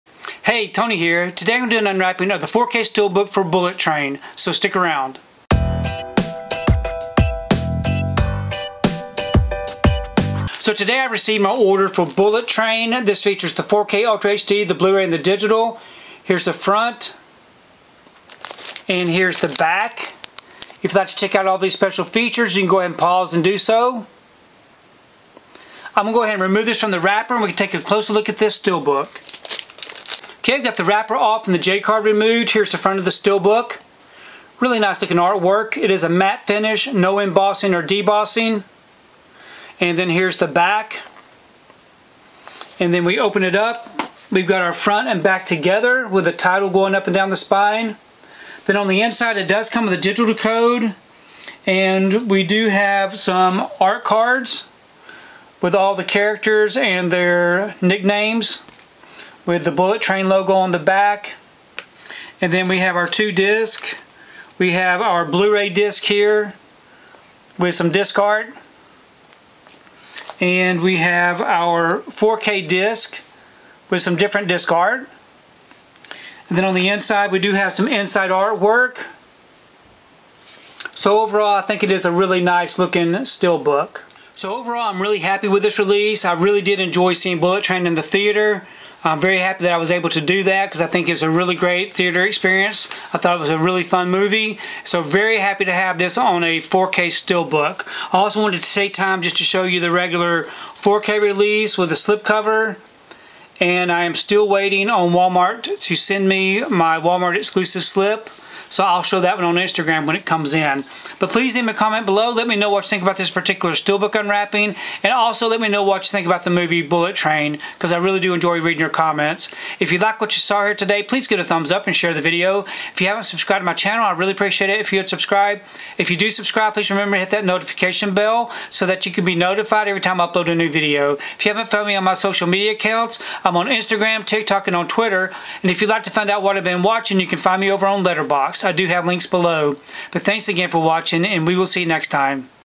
Bullet Train 4K Ultra HD + Blu-ray Steelbook Unwrapping
Check out my unwrapping of the Bullet Train 4K Ultra HD + Blu-ray Steelbook!

► Purchase the 4K Ultra HD
► Purchase the 4K Ultra HD Steelbook
► Purchase the Blu-ray